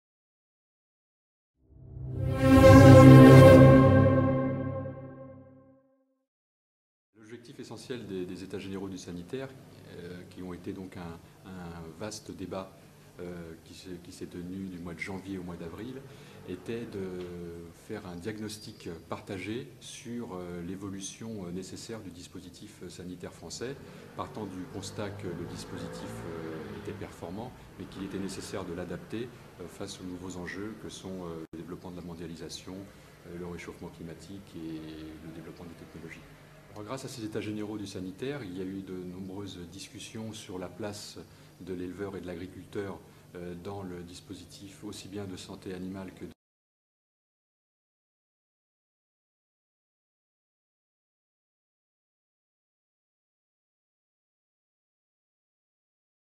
Nouvelle politique sanitaire : « Reconnaissance accrue des compétences des agriculteurs »
Jean-Luc Angot, directeur général adjoint de l'Alimentation, revient à l'issue des Etats-Généraux du sanitaire clôturés le 16 septembre 2010 sur les objectifs du nouveau dispositif en France, et ses implications pour les agriculteurs. Parmi les actions prévues, la redéfinition de l'acte vétérinaire figure en bonne place. Elle permettra aux éleveurs la réalisation de certains soins à leurs animaux. Les modalités seront bientôt fixées par décret. Au sujet de la surveillance du territoire, il est prévu de créer une plate-forme d'épidémiosurveillance au niveau national et de la décliner localement.